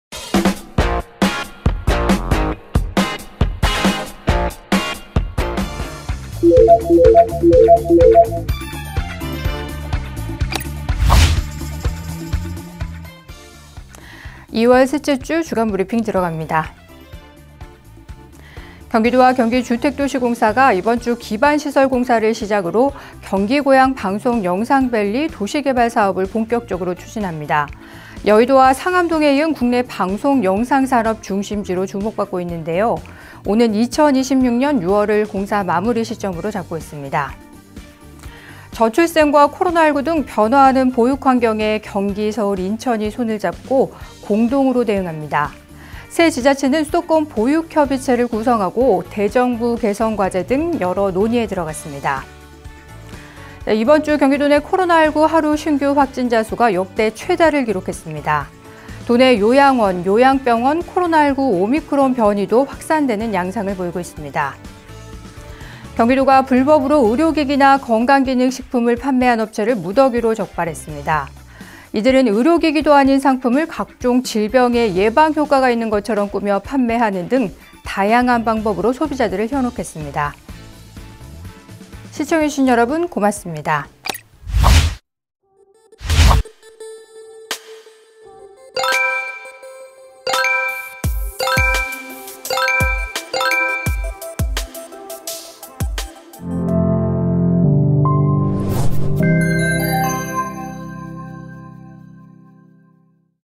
2월 셋째 주 주간브리핑
2월 셋째 주 주간브리핑

[자막]1. 경기고양 방송영상밸리 착공..2026년 마무리
경기도와 경기주택도시공사가 이번 주 기반시설 공사를 시작으로 ‘경기고양 방송영상밸리 도시개발사업’을 본격적으로 추진합니다. 여의도와 상암동에 이은 국내 방송‧영상산업 중심지로 주목받고 있는데요. 오는 2026년 6월을 공사 마무리 시점으로 잡고 있습니다.

[자막]2. 보육정책, 수도권이 공동대응
저출생과 코로나19 등 변화하는 보육환경에 경기-서울-인천이 손을 잡고, 공동으로 대응합니다. 세 지자체는 ‘수도권 보육협의체’를 구성하고, 대정부 개선과제 등 여러 논의에 들어갔습니다.

[자막]3. 하루 확진자 최다·요양시설 오미크론 확산
이번 주 경기도내 코로나19 하루 신규 확진자수가 역대 최다를 기록했습니다. 도내 요양원·요양병원 코로나19 오미크론 변이도 확산되는 양상을 보이고 있습니다.

[자막]4.불법 의료기기 판매 업체 무더기 적발
경기도가 불법으로 의료기기나 건강기능식품을 판매한 업체를 무더기로 적발했습니다. 이들은 의료기기도 아닌 상품을 각종 질병에 예방 효과가 있는 것처럼 꾸며 판매하는 등, 다양한 방법으로 소비자들을 현혹했습니다.

-반려동물테마파크 다음달 준공..상반기 개장
-중소·벤처 신산업 전환 도울 ‘경기 구조혁신지원센터’ 개소
-경기도 초등학생 2만5천명에 ‘안전우산’
-평생학습포털 지식, 가상자산 등 81개 신규강좌 개설
-탄소중립·생산성 중점..축산분야 3천억원 투입